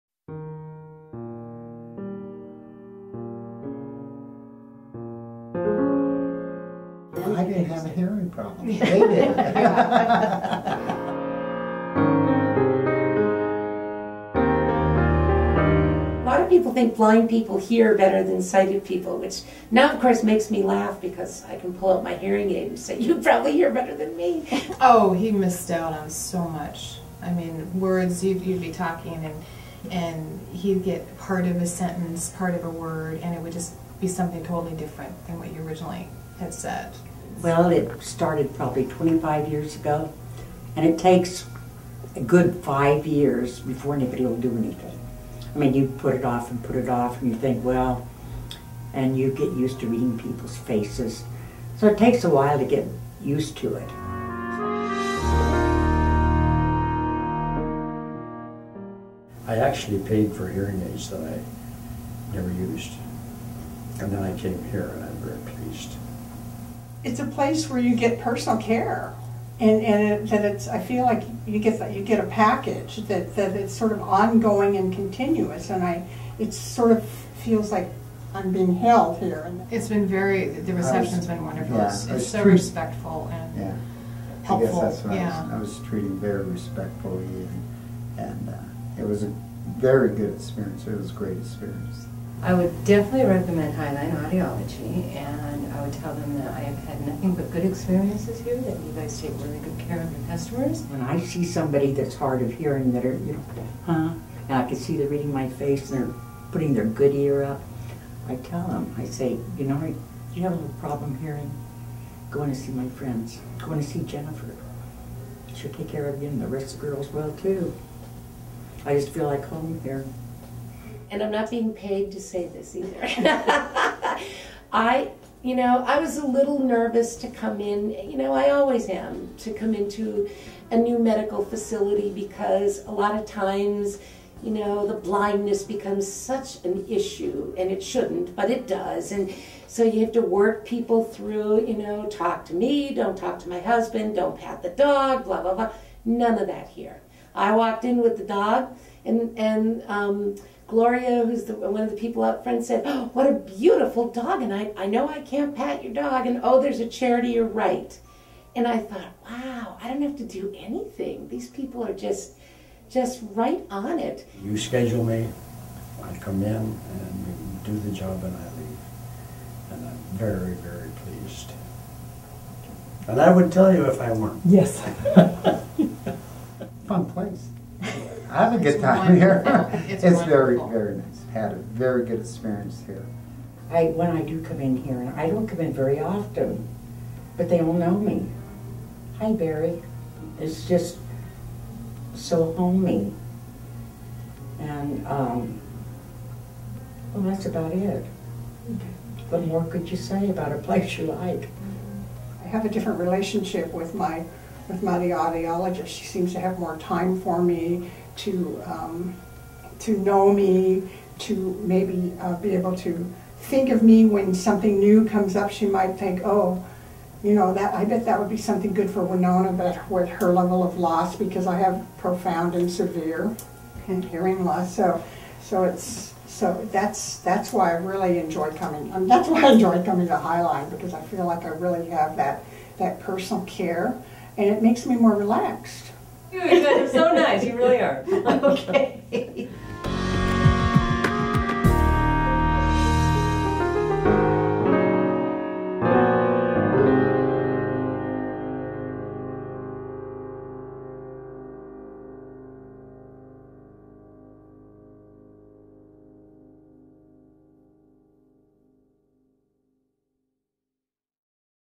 Why Go To Highline Audiology?
Testimonial video from non-paid, real people who are patients at Highline Audiology in Burien, WA. We care for your hearing.